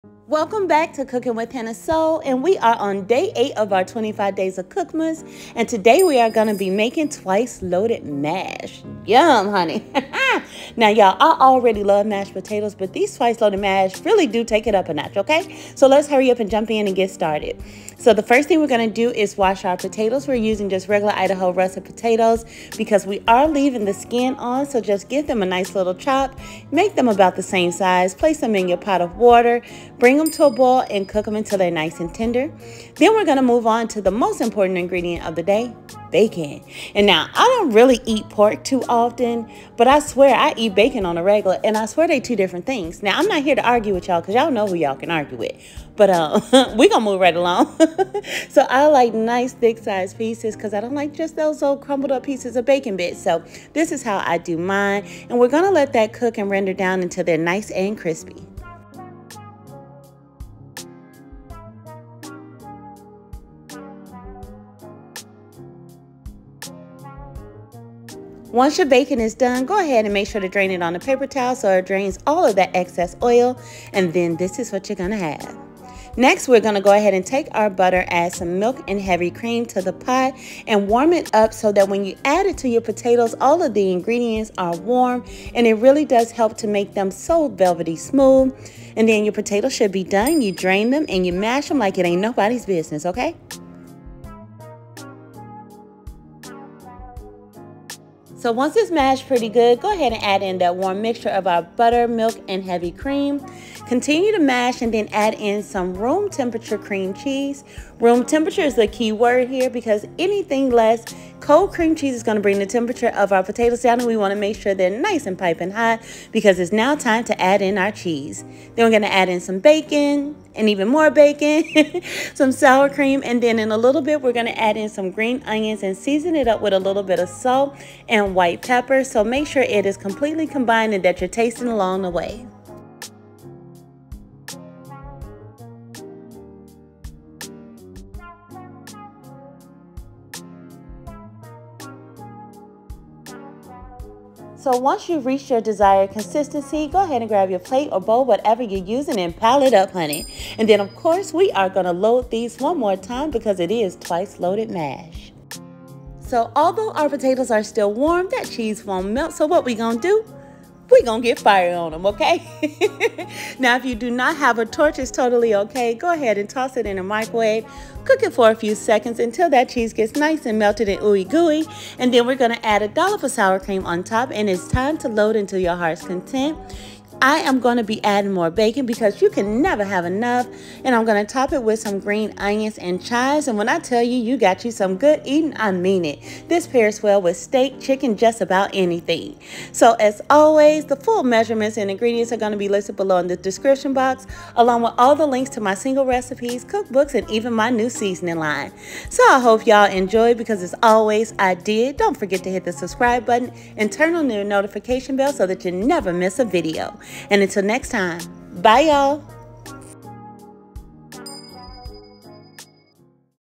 Cookmas Day 8 | Twice Loaded Mash | Best Mashed Potato Recipe
Full recipe:
8-10 russet potatoes
6 slices of thick cut bacon
1/2 cup milk
1/3 cup heavy cream
4 tablespoons butter
2 tablespoons cream cheese (room temperature)
1 cup cheddar cheese
1/3 cup sour cream
Green onions
Chives
Salt
White pepper

NEW SEASONING LINE!!!

PURCHASE MY NEW HARDCOVER COOKBOOK AT THE LINK BELOW, 50 FULL RECIPES!

Hint of Soul
3801 S. Central Ave.